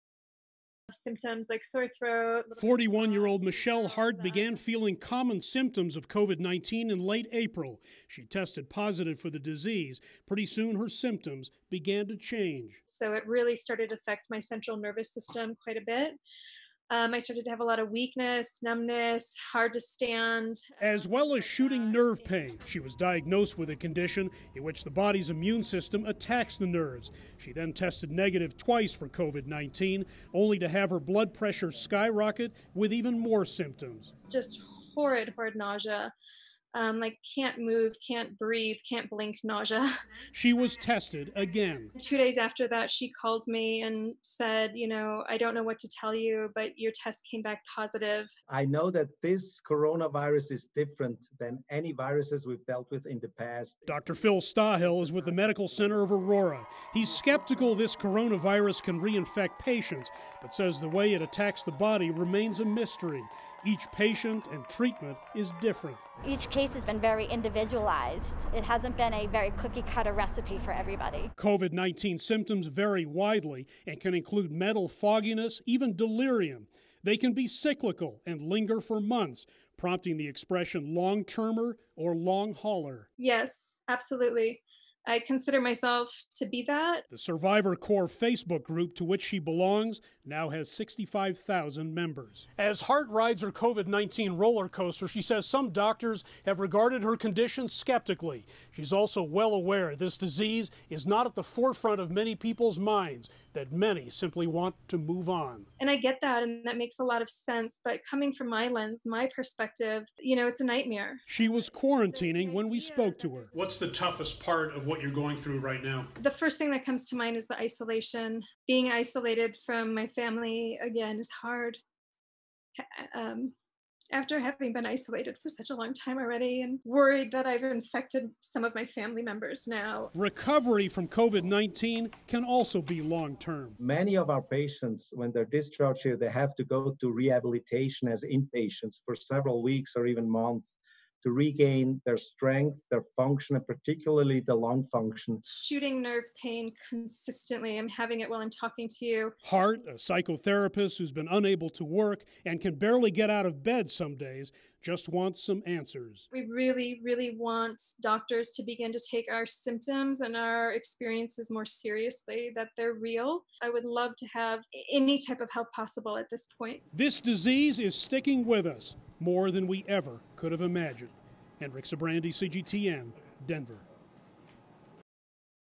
Who are the “long haulers?” The mental toll of COVID-19
Britain has announced it’s putting over $10 million into a new study to examine the long-term impact of COVID-19 on patients.

The disease causes not just severe respiratory issues. It can also produce a variety of physical and mental challenges that are very hard to shake. That’s given rise to the term “long haulers.”